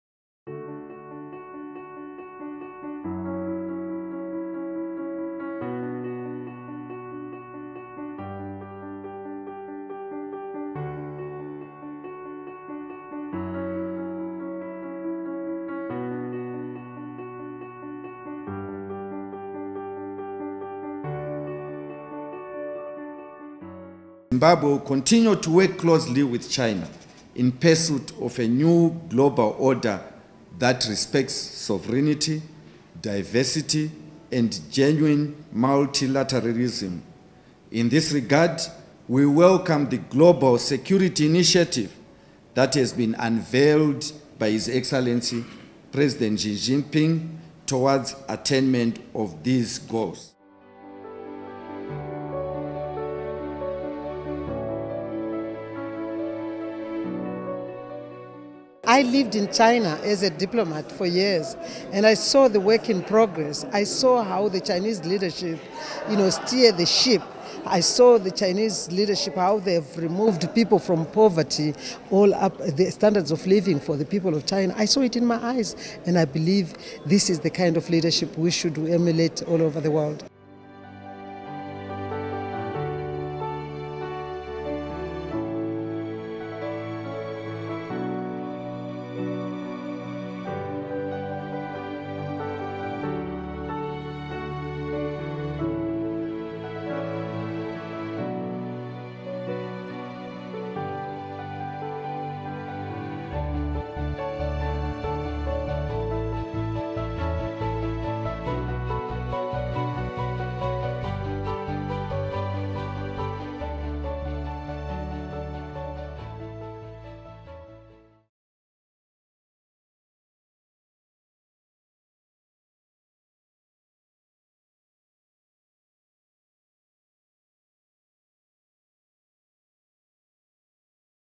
Chinese Embassies in Africa Host Receptions to Celebrate 73rd Founding Anniversary of PRC
Chinese embassies in countries across Africa held receptions to celebrate the 73rd anniversary of the founding of the People's Republic of China (PRC) which falls on Saturday this year.